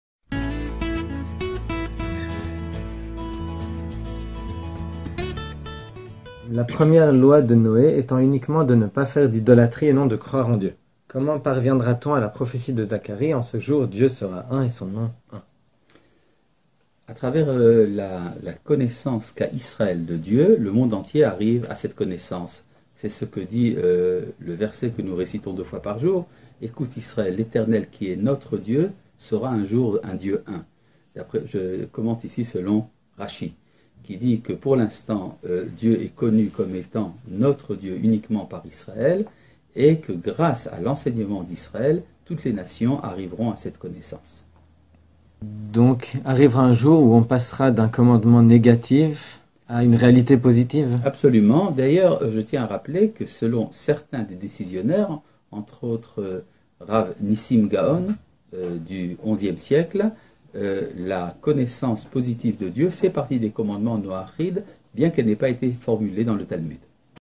1e Loi de Noé: pas d'idolâtrie ou croire en Dieu?
La 1e loi de Noé étant uniquement de ne pas faire d'idolâtrie et non de croire en Dieu, comment parviendra-t-on à la prophétie de Zacharie: en ce jour Dieu sera Un et son Nom Un?

Soutenez le Projet Noachide et ensemble nous allons rendre le monde meilleur